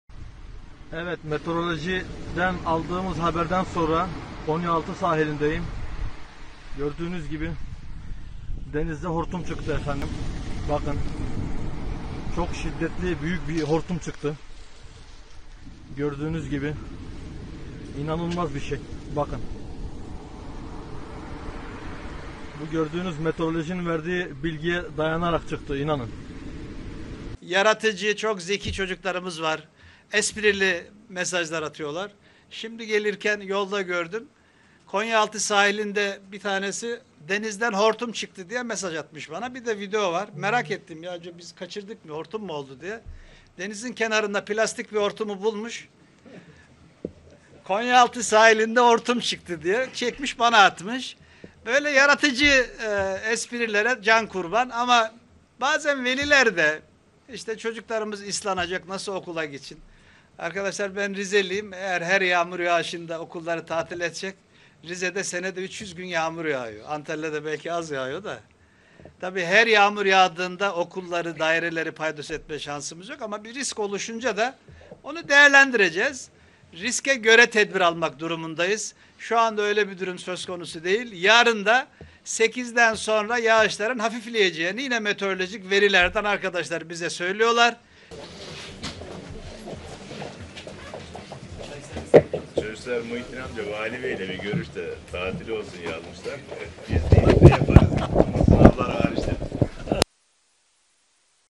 Vali'yi güldüren 'hortum' videosu! 'Böyle esprilere can kurban'
Kırmızı kod uyarısı yapılan Antalya'da, bir vatandaşın Konyaaltı Sahili’nde plastik boruyu eline alıp ‘hortum çıktı’ diye yaptığı video paylaşımı, sosyal medyada gündem oldu. Antalya Valisi’nin de dikkatini çeken paylaşıma ilişkin Vali Karaloğlu, “Videoyu bana atmış. Hortum mu çıktı diye merak ettik. Böyle esprilere can kurban” dedi.